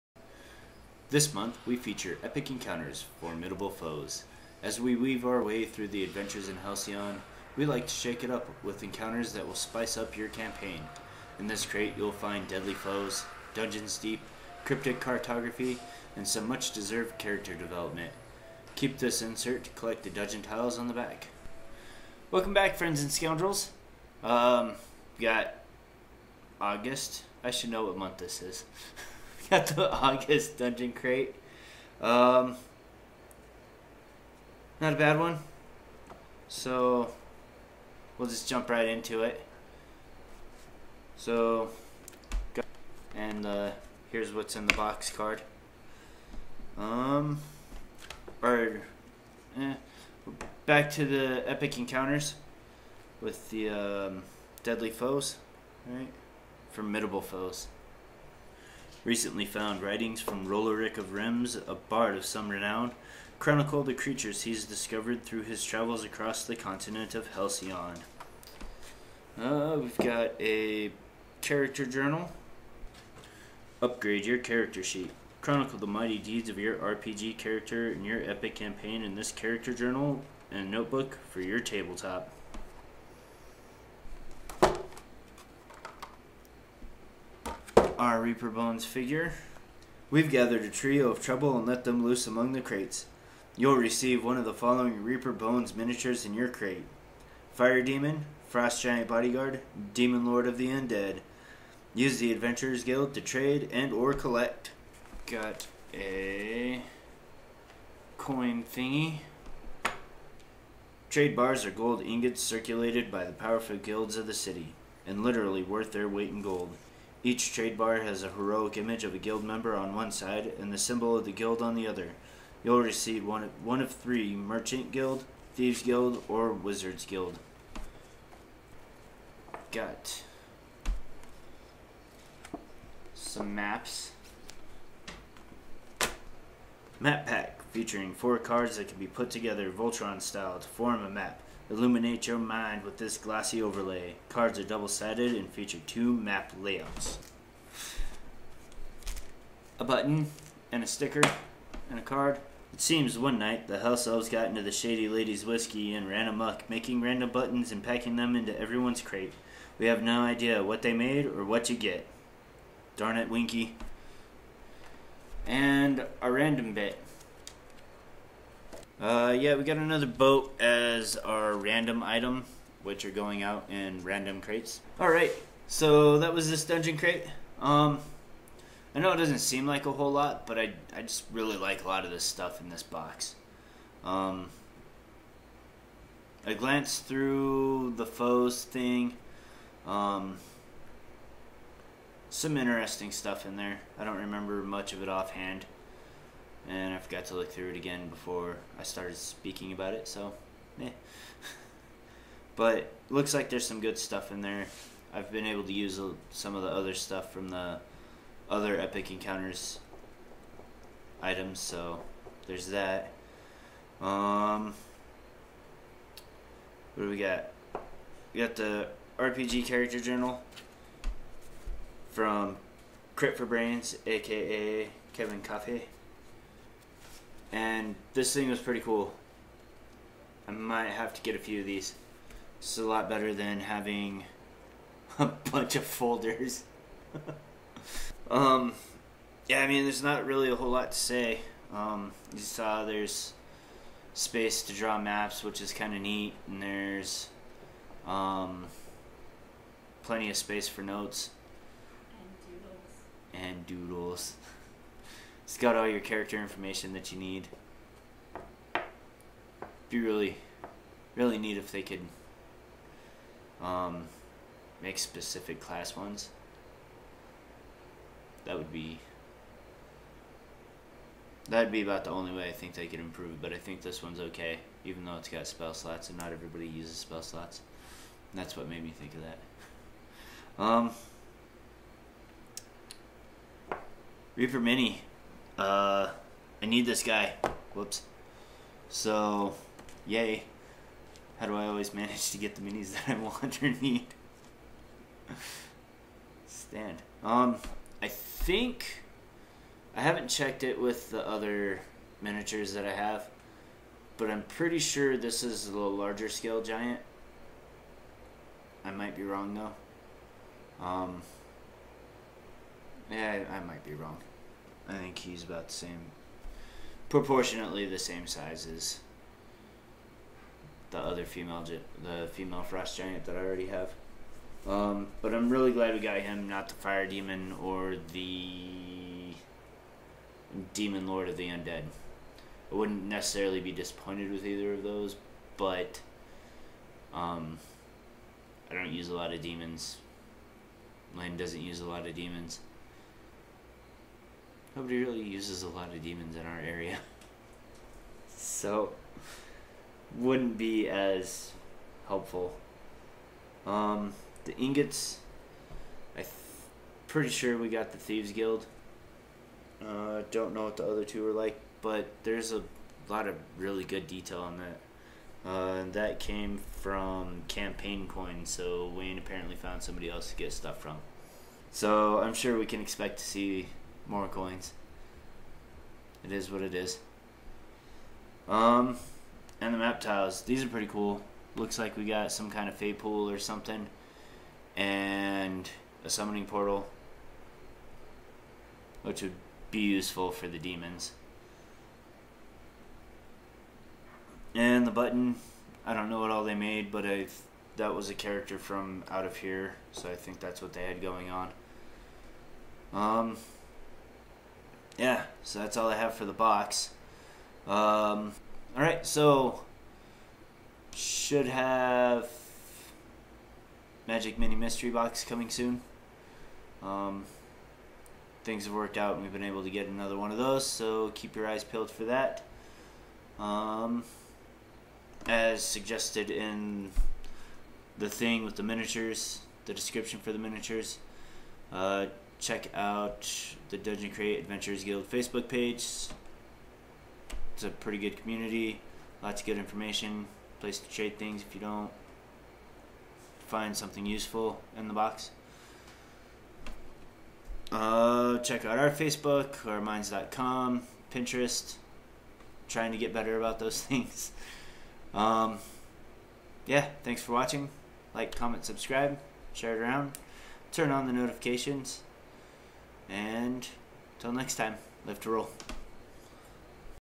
Unboxing Dungeon Crate: August 2018
Join us as we unbox the August 2018 Dungeon Crate: Formidable Foes.

Links to the Dungeon Crate, theDungeon Crate Adventurer's Guild and of course websites for the contributors for this box:

Contributors:
Reaper-